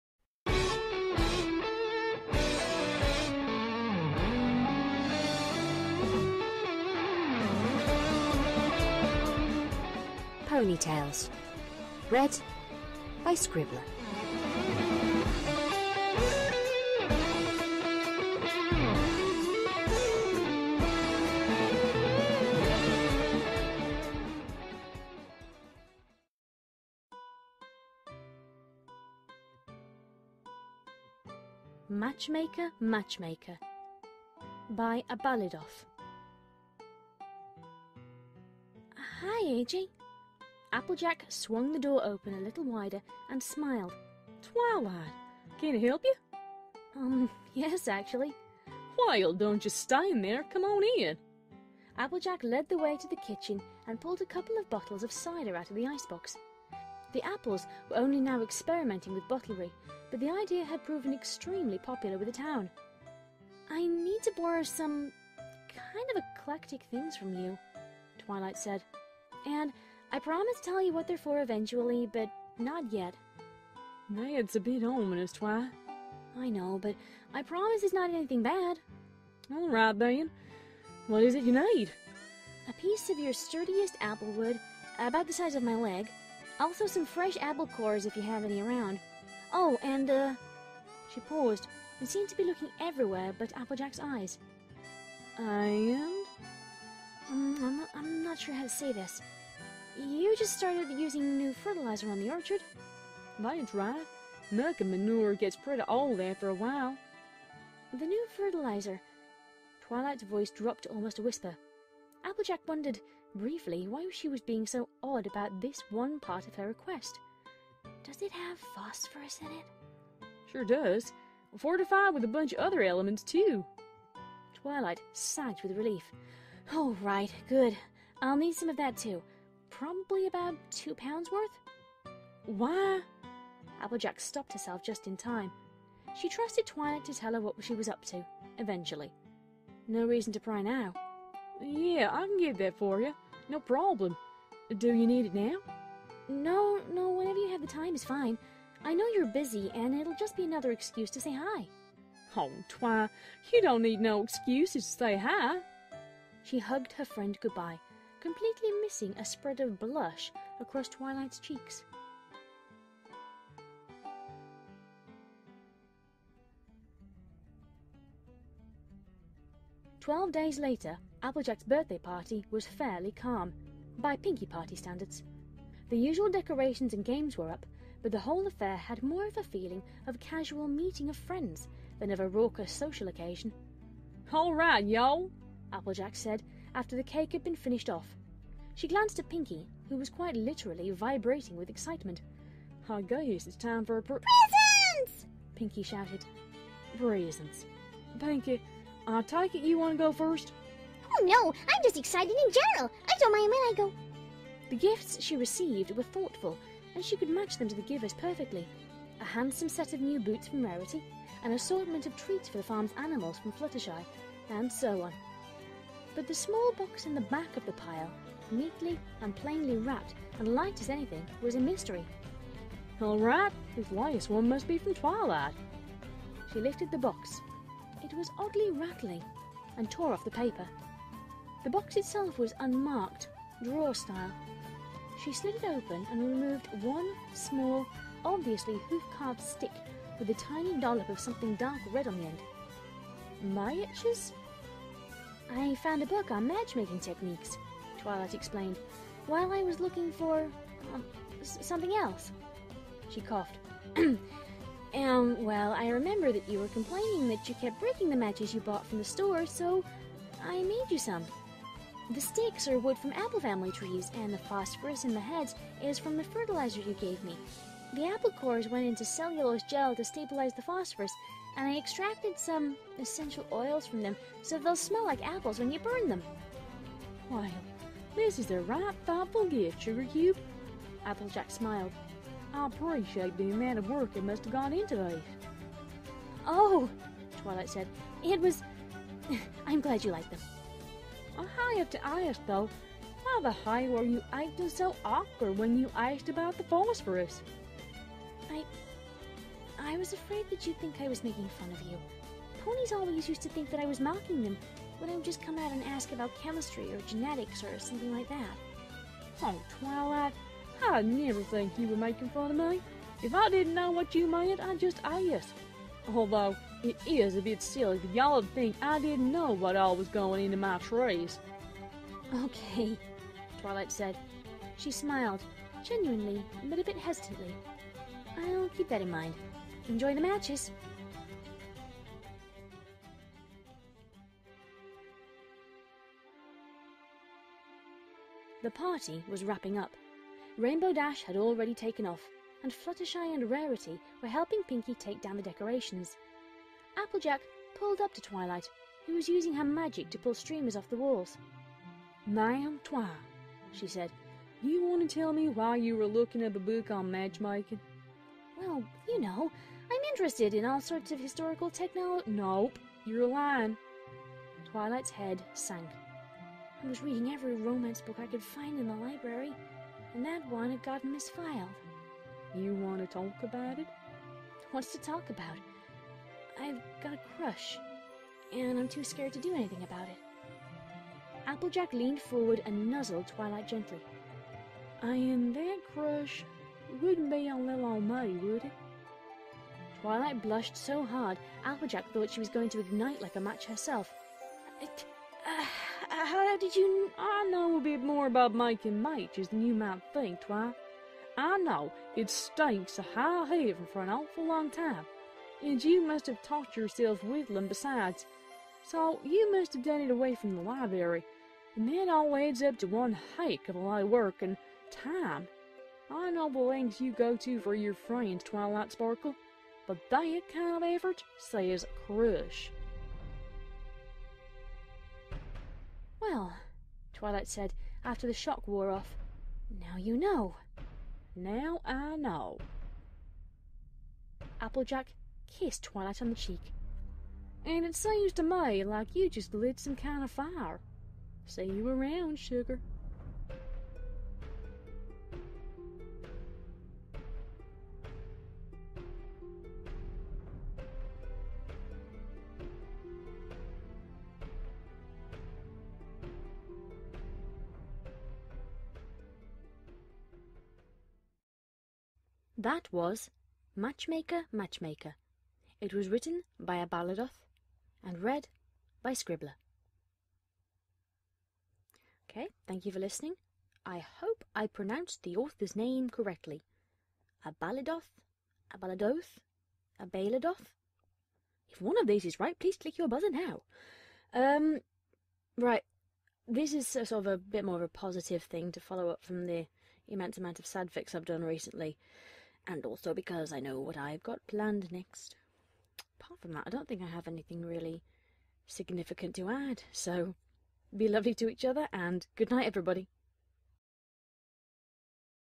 "Matchmaker Matchmaker" A Fanfic Reading By Scribbler | MWR Gameplay
This is a fanfic not read by me as I do not have a proper mic. I take readings that have no visual interest and add visualization.

Summary: Twilight finds a book on matchmaking.

Downloadable .mp3 versions of Pony Tales Fanfic Readings can be found

Art Used:

Meadow Stroll by honey_laurel17